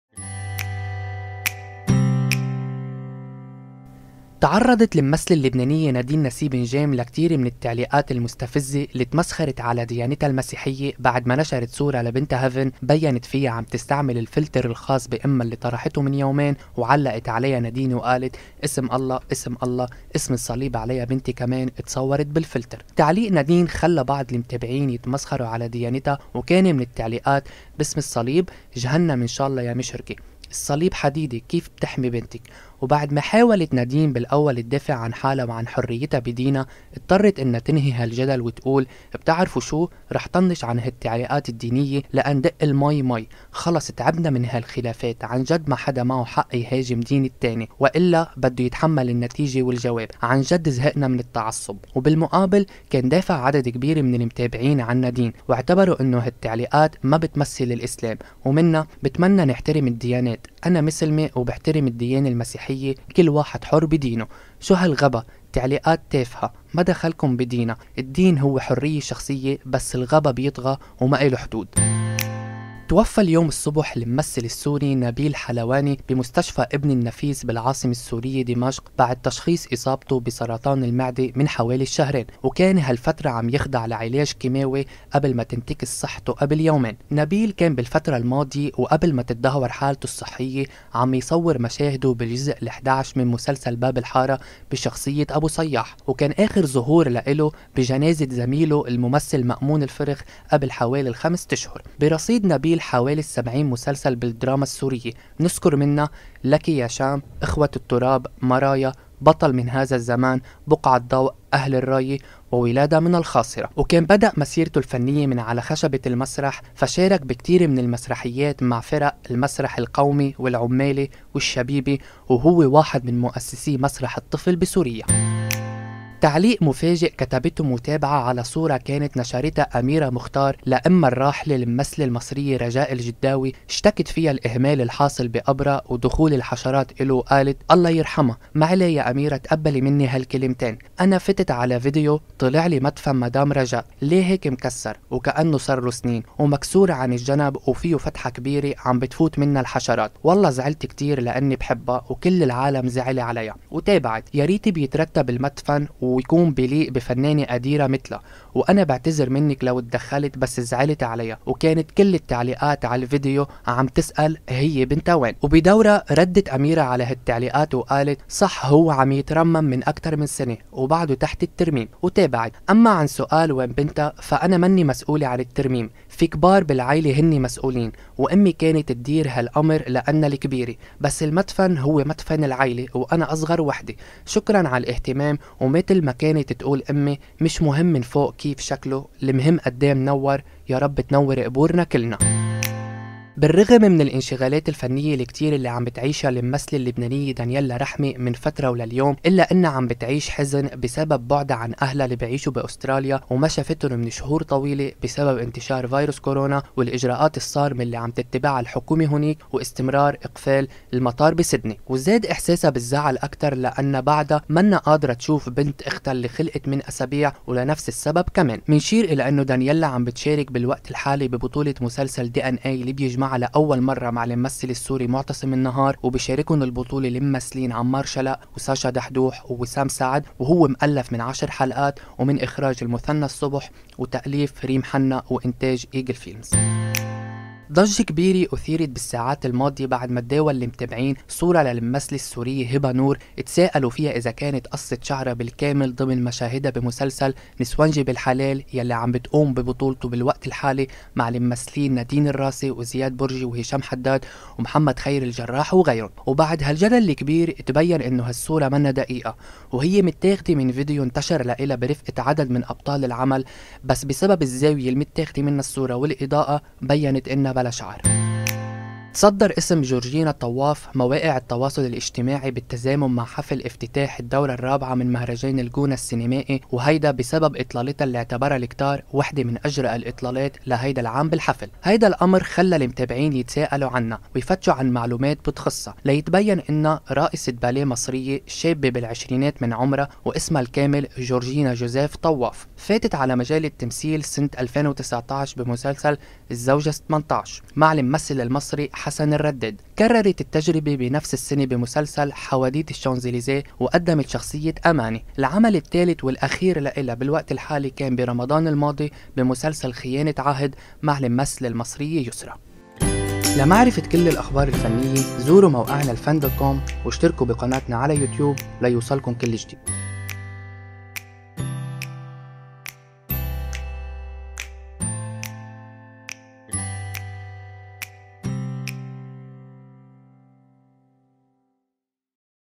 نادين نسيب نجيم بقرار مفاجئ بسبب السخرية من ديانتها..وفاة ممثل "باب الحارة" ومن هي جورجينا طواف؟
نادين نسيب نجيم ترد بطريقتها الخاصة على الساخرين من ديانتها.. بالصورة
متابعة تشكو دخول الحشرات قبر رجاء الجداوي وإبنتها تردّ-بالصورة
هبة نور تثير الجدل بسبب لقطة ظهرت فيها حليقة الرأس في "نسونجي بالحلال"-بالفيديو
تعرفوا إلى جورجينا طواف التي أثارت الجدل بإطلالتها في مهرجان الجونة

Copyright : ElFann.com